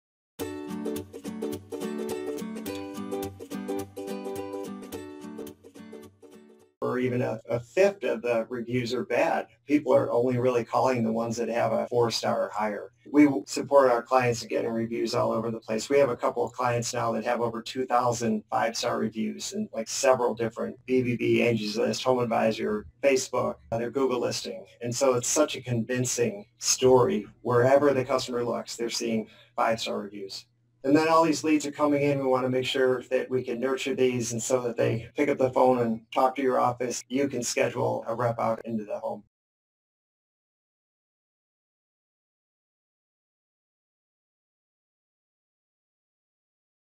Customer Reviews Are VITAL | Rainmaker For Contractors
Customer Reviews can MAKE or BREAK your business. Bill talks about how customer reviews can influence lead generation and more.